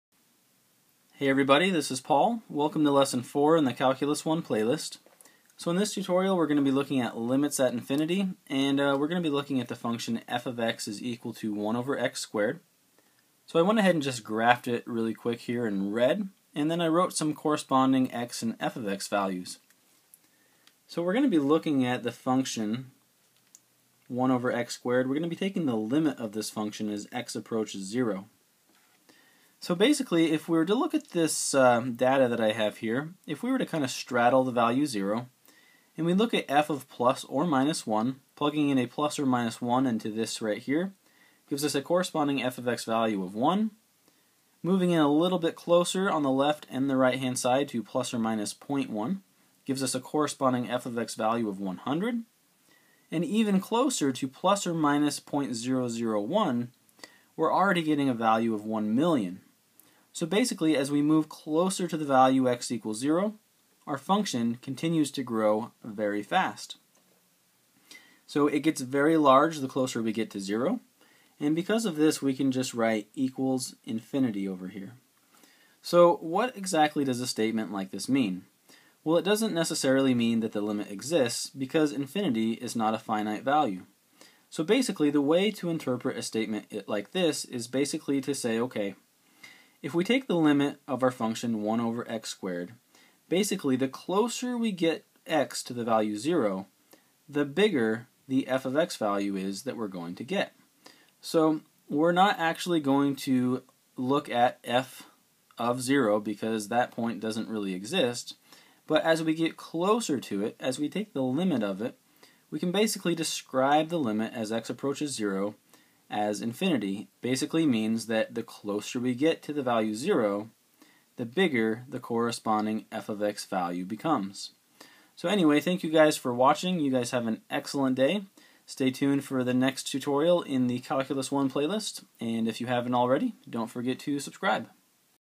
Limits at Infinity , Calculus 1 , Lesson 4
Here, I explain a limit at infinity.